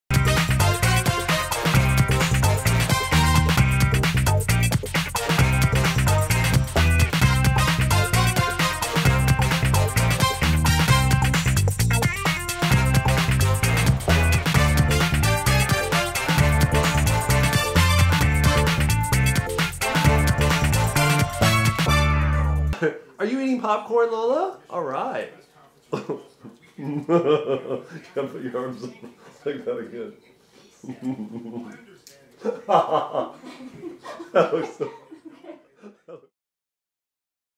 Lola Loves Popcorn
Shih-Poo dog (Lola) loves eating popcorn with her people hands